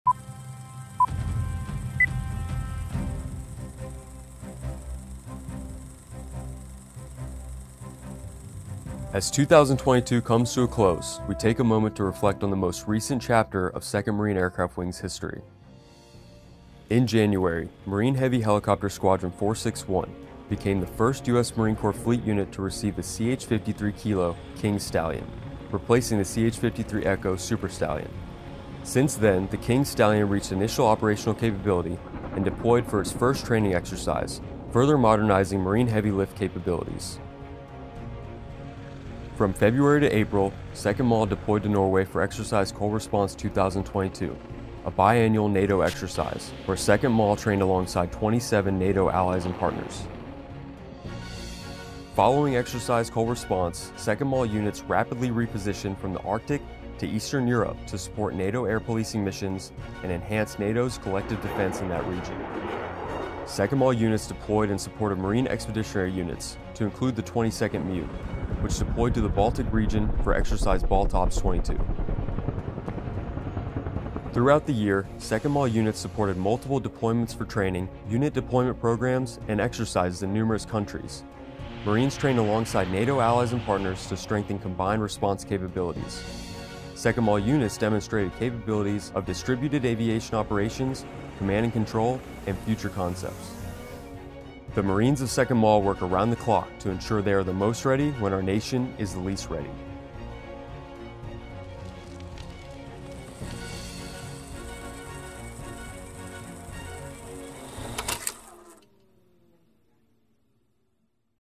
Battles Won Since 1941: 2nd MAW's Year In Review
Video production of U.S. Marines with 2nd Marine Aircraft Wing (MAW) supporting the Marine Air-Ground Task Force throughout the world during 2022.

MARINE CORPS AIR STATION CHERRY POINT, NC, UNITED STATES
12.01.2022

2nd MAW is ready and capable to deploy around the world in a moment’s notice in defense of the nation and its allies and partners. 2nd MAW is the aviation combat element of II Marine Expeditionary Force. (U.S. Marine Corps video by 1st Lt. Hudson Sadler, Cpl. Caleb Stelter, Cpl. Christian Cortez, Cpl. Adam Henke, and Pfc. Rowdy Vanskike)

Film Credits: Video by Sgt. Servante Coba, Cpl. Christian Cortez, Cpl. Adam Henke, Cpl. Christopher Hernandez, Lance Cpl. Elias Pimentel, 1st Lt. Hudson Sadler, Cpl. Lauren Salmon, Lance Cpl. Anakin Smith, Cpl. Caleb Stelter and Pfc. Rowdy Vanskike
2nd Marine Aircraft Wing